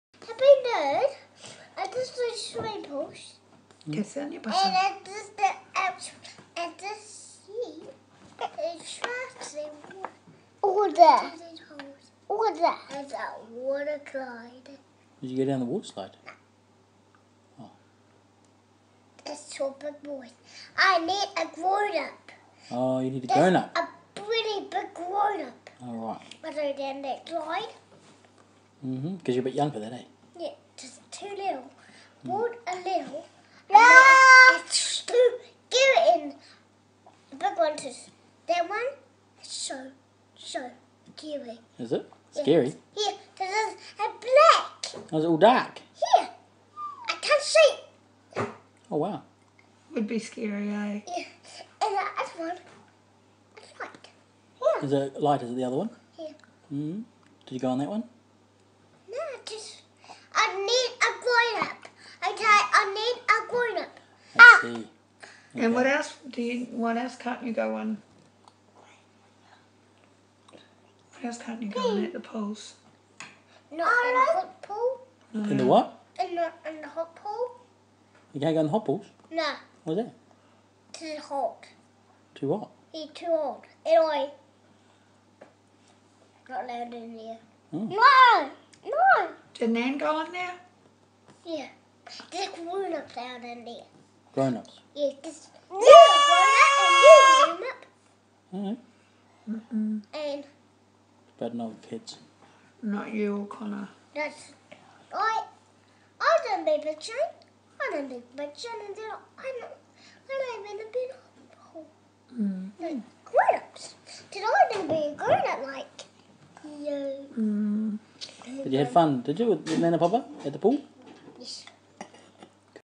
3 year old explains: Why no water slide?
My son explains why he couldn't go down the hydro slide.